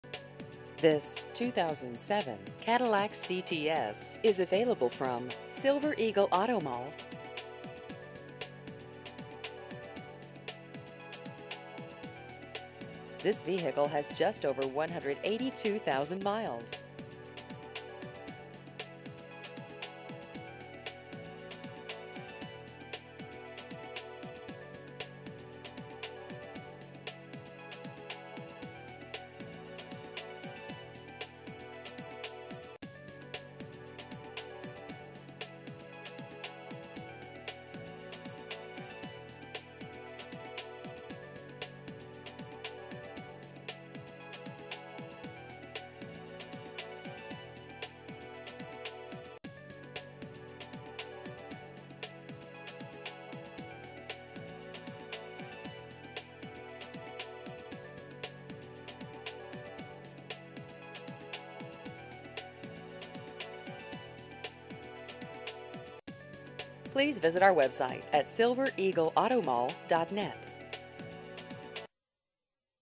2007 Cadillac CTS Used Cars Kansas City MO
This 2007 Cadillac CTS is available from Silver Eagle Auto Mall.